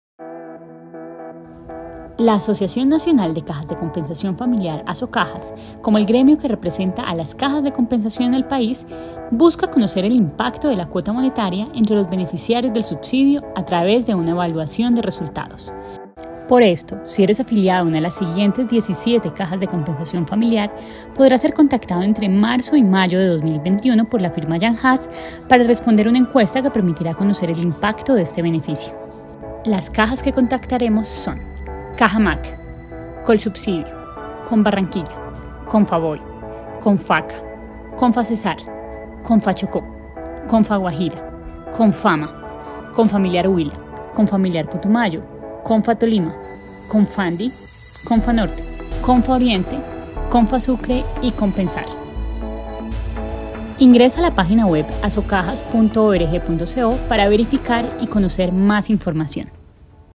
Cuota Monetaria | Evaluación de resultados del subsidio.
Nuestro objetivo es conocer el impacto que genera la CuotaMonetaria entre los beneficiarios del subsidio 🤓 Es por ello que entre marzo y mayo podrías ser contactado por la firma Yanhaas S.A. para ser parte de la evaluación de resultados📋 Conoce en el siguiente vídeo las 17 CajasDeCompensaciónFamiliar que serán contactadas.